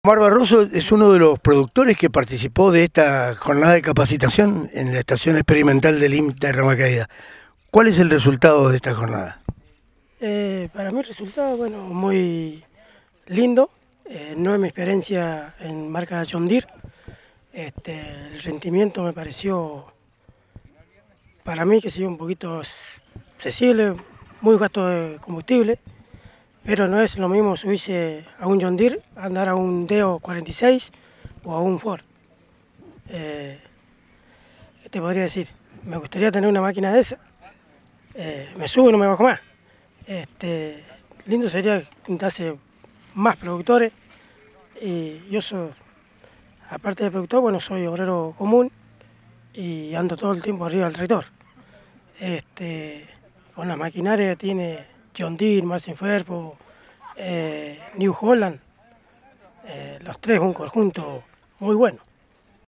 Jornada de Capacitacion INTA Rama Caída San Rafael (3a.Parte)
CN10TV San Rafael entrevista a Omar Barroso productor participante de la capacitación realizada a productores y obreros en INTA Rama Caida San Rafael Mendoza